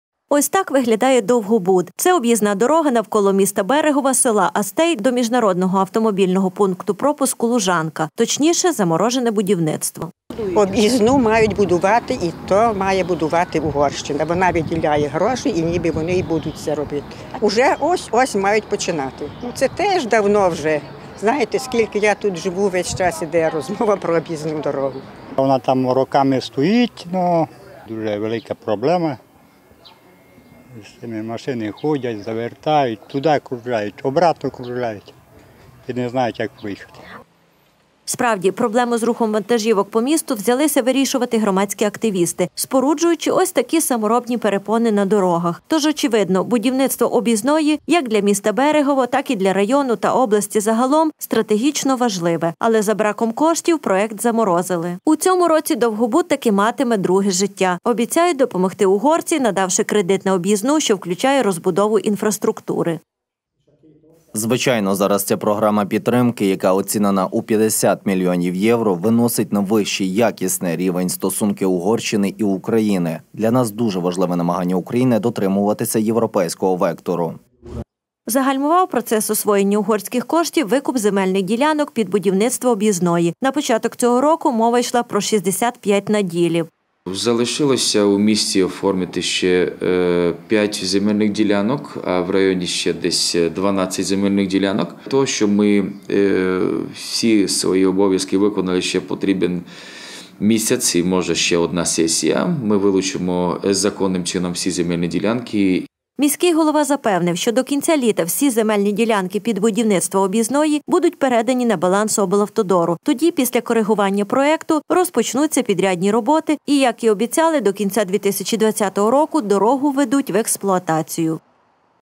Гальмування будівництва Берегівської об’їзної
Ось так виглядає довгобуд - це об'їзна дорога навколоміста Берегво, села Астей до міжнародного автомобільного пункту пропуску «Лужанка». Точніше заморожене будівництво.Справді проблему з рухом вантажівок по місту взялися вирішувати громадські активісти, споруджуючи ось такі саморобні перепони на дорогах. То ж очевидно - будівництво об’ізної їяк для міста Берегово, так і для району та області загалом стратерічно важливе. Але за браком коштів проект заморозили. У цьому році довгобуд таки матиме друге життя, обіцяють допомогти угорці, надавши кредит на об’їзну, що включає розбудову інфраструктури.Загальмував процес освоєння угорських коштів викуп земельних ділянок під будівництво об’їзної. На початок цього року мова йшла про 65 наділів.Міський голова запевнив, що до кінця літа всі земельні ділянки під будівництво об’їзної, будуть передані на баланс облавтодору. Тоді після корегування проекту, розпочнуться підрядні роботи і, як і обіцяли, до кінця 2020 року дорогу введуть в експлуатацію.